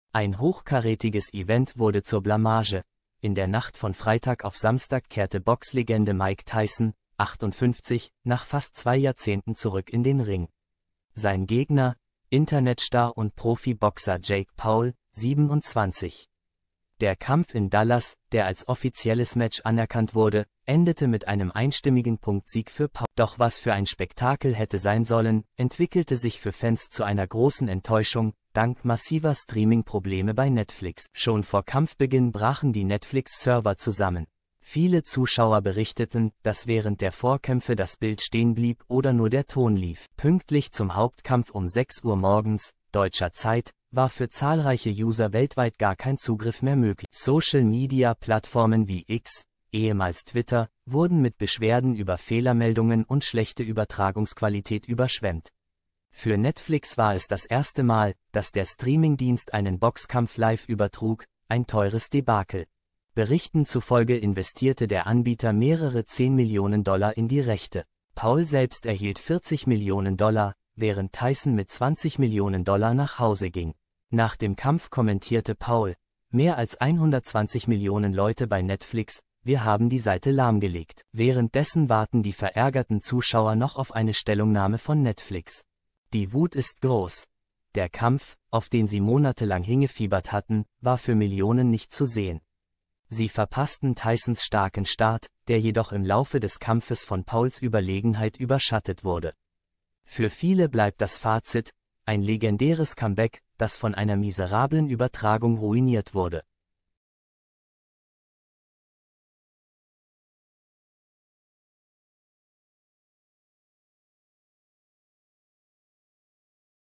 Server brechen zusammen ++ Millionen Zuschauer stinksauer:Das Netflix-Desaster beim Tyson-Kampf
Das große Comeback von Mike Tyson endet in einem Skandal! Beim offiziellen Boxkampf gegen Jake Paul erlitt die Legende nicht nur eine Niederlage, sondern auch Netflix erlebte ein Desaster. Server brachen zusammen, Millionen Zuschauer konnten den Kampf nicht sehen und beschwerten sich weltweit über Übertragungsprobleme. Erfahren Sie, was wirklich geschah, warum Fans so wütend sind und wie Jake Paul nach seinem Sieg triumphierte. Verpassen Sie nicht die neuesten Reaktionen und Details zum Mega-Event!